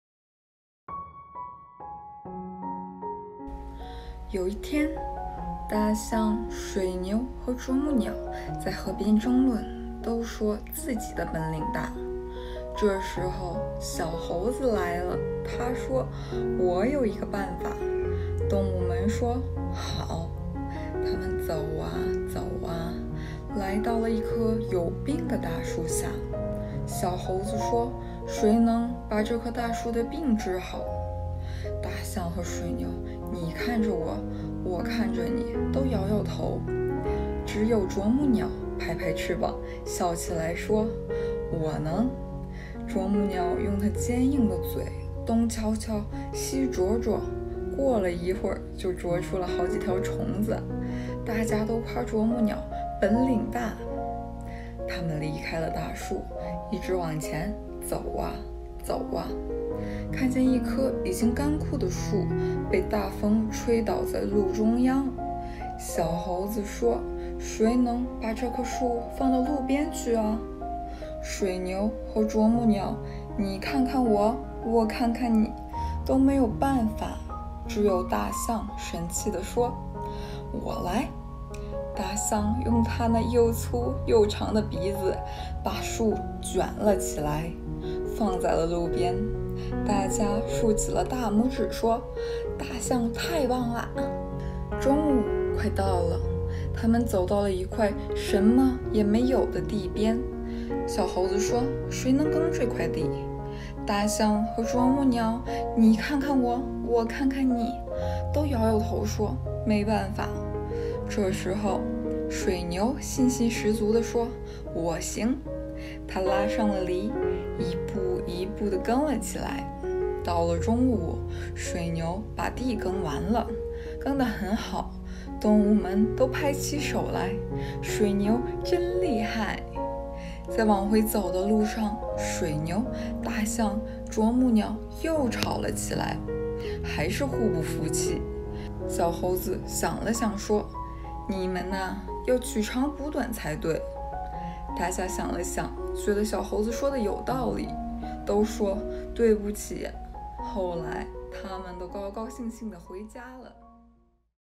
Chinese bedtime story - 谁的本领大－Learn while you sleeping- ASMR help you to sleep
Practice listening while sleeping.
Full story:
有一天，大象、水牛和啄木鸟在河边争论，都说自己的本领 大。这时候，小猴子来了，它说：“我有一个办法。”动物们说：“好。”

它们走啊走啊，来到了一棵有病的大树下，小猴子说：“谁能把这棵大树的病治好?”大象和水牛你看着我，我看着你，都摇摇头，只有啄木鸟拍拍翅膀，笑起来说：“我能。”啄木鸟用它坚硬的嘴东敲敲，西啄啄，过了一会儿就啄出了好几条虫子。大家都夸奖啄木鸟本领大。

它们离开了大树，一直往前走啊走啊，看见一棵已经干枯的树被大风吹倒在路中央。小猴子说：“谁能把这棵树放到路边去啊?”水牛和啄木鸟你看看我，我看看你，都没办法，只有大象神气地说：“我来。”大象用它那又粗又长 的鼻子把树卷了起来，放在路边，大家竖起了大拇指说：“大象太棒了。”

中午快到了，它们走到一块什么也没有的地边，小猴子说：“谁能耕这块地?”大象和啄木鸟你看看我，我看看你，都摇摇头说：“没办法。”这时候，水牛信心十足地说：“我行。”它拉上了犁，一步一步地耕起地来。到了中午，水牛把地耕完了，耕得很好，动物们都拍起手来：“水牛真厉害。”

在往回走的路上，水牛、大象、啄木鸟又吵起来了，还是互不服气。小猴子想了想说：“你们呢，要取长补短才对 !”大家想了想，觉得小猴子说的有道理，都说：“对不起。”后来，它们都高高兴兴地回家了。